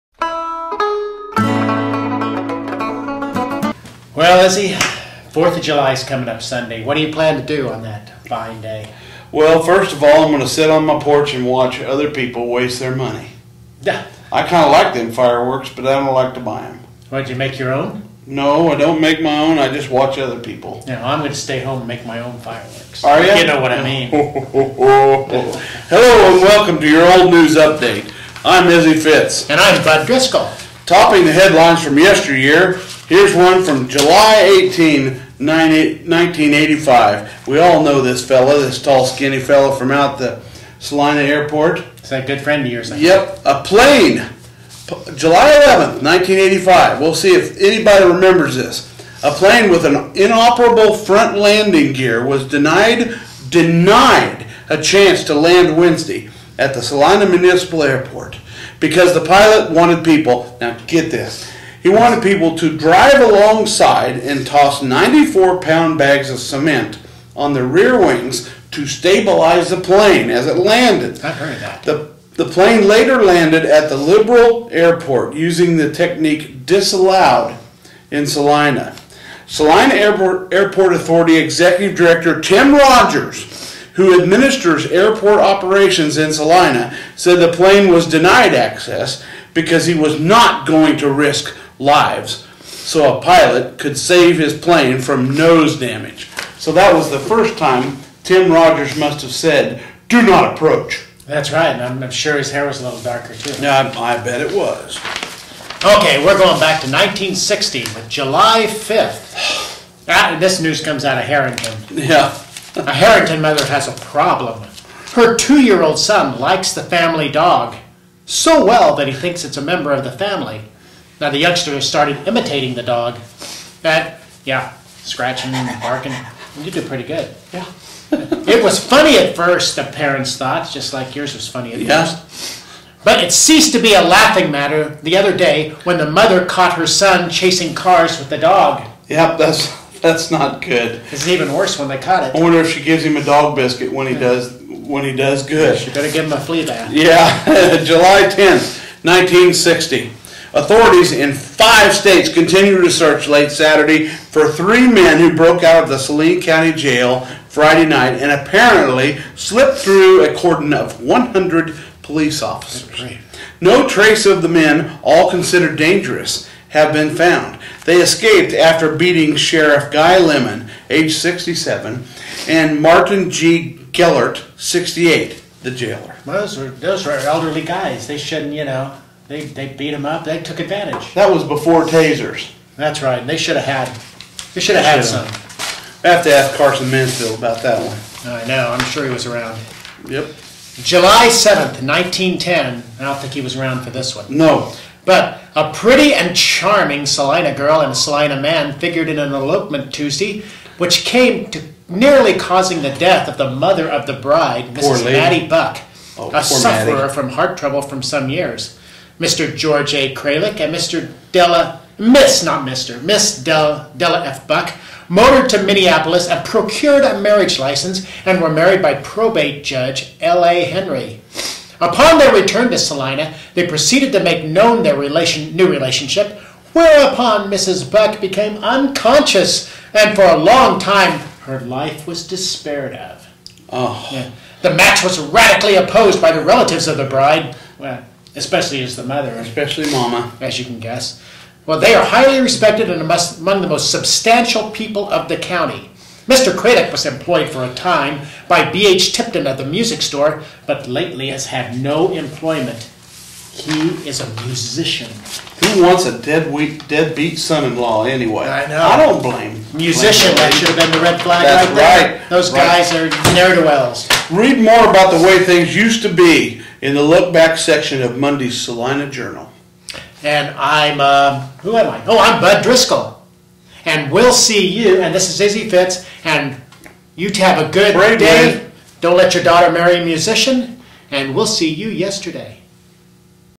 Yesternews with Bud and Izzy
Bud and Izzy discuss 1985 plane problems at the Salina Municipal Airport, escaped convicts from the Saline County Jail in 1960, a 1910 eloped couple that led to family problems.